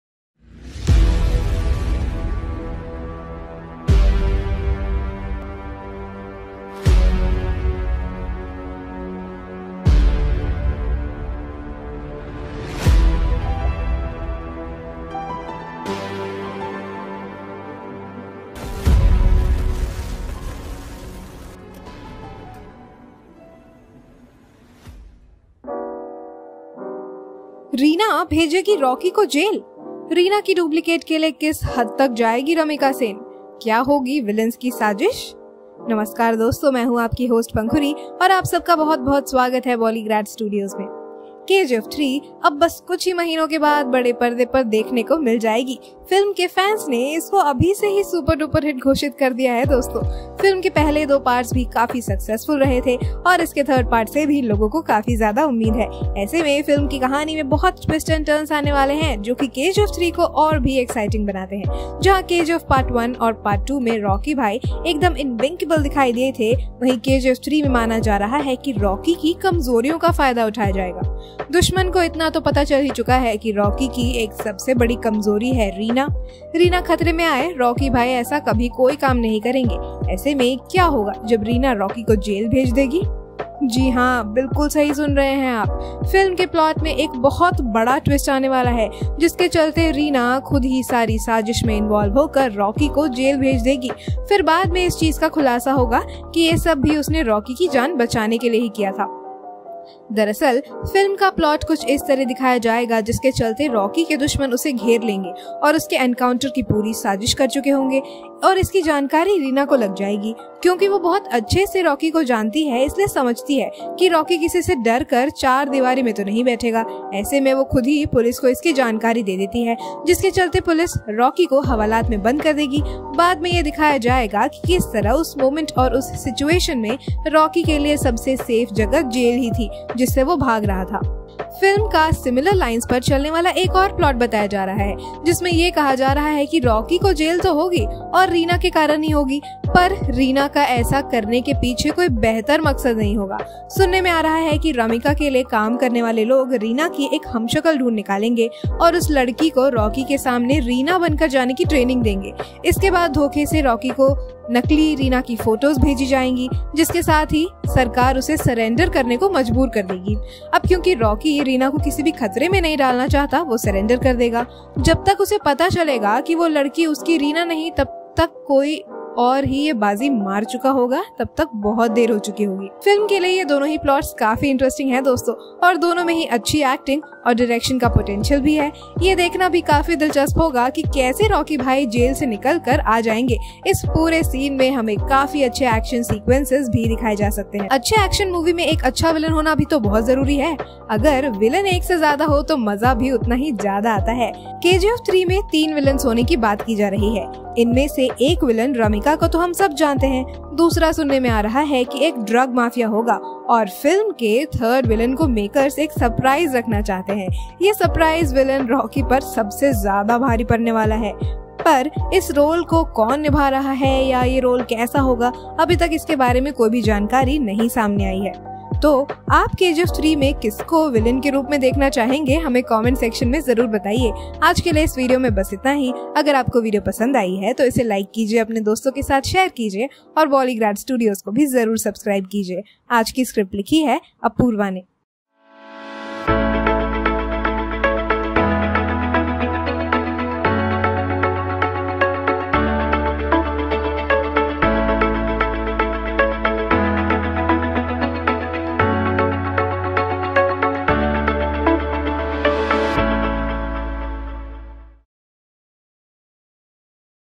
KGF Chapter 3 | 41 Aamazing Facts | Yash | Sanjay Dutt | Raveena Tandon| Prakash | Prashanth Neel
KGF director Prashanth Neel: 'Definitely a possibility of KGF Chapter 3, that also comes out of compulsion'

0:00 intro
1:00 Reena भेजेगी Rocky को jail?I
1:20 Reena की duplicate के लिए किस हद तक जाएगी Ramika sen?
2;00 क्या होगी villains की साजिश?

KGF director Prashanth Neel has said that the success of the first two films have put a ‘compulsion’ to make the third part. He added that ‘we don’t know when, but we will keep it going'.

KGF: Chapter 2 is a Kannada film, starring Yash and is a sequel to the 2018 hit KGF: Chapter 1. KGF Chapter 2 has earned over ₹1250 crore at the box office, and ₹435 crore of those came from the Hindi version alone.

Asked whether he was actually considering KGF Chapter 3, Prashanth told the Youtube channel Galatta Plus, "There is definitely a possibility of Chapter 3. That also comes out of compulsion. People have loved this world, they've loved this character and we are gonna keep it going. We don't know when, but we are gonna keep it going".

He added, "Of course, we have an idea. We had an idea for it a long time back actually, but right now we just want to take a big break and we will definitely come back to make that."

KGF Chapter 3, its making and release: Know what Neel said

Everyone must have it in their memory that the post-credit sequence of KGF Chapter 2 gave a glance of a file named KGF Chapter 3 final draft when Rocky's crimes in the United States of America were being investigated following his 'death' in the climax of the second installment.

When asked if the glance was just a tease or if there is a possibility of the third installment, Neel said, "There is definitely a possibility of Chapter 3. That also comes out of compulsion. People have loved this world, they've loved this character and we are gonna keep it going. We don't know when, but we are gonna keep it going".

He further said, "We had an idea for it for a long time but right now we just wanna take a big break from KGF and we will definitely come back to make that."

Before spilling the beans about KGF 3, Neel also revealed some of the unknown things that the fans were anxious about.

Voice - Pankhuri
Script by - Apoorva
Editor- Shrutaparna Paul

Thumbnail que
क्या Rocky को होगी jail?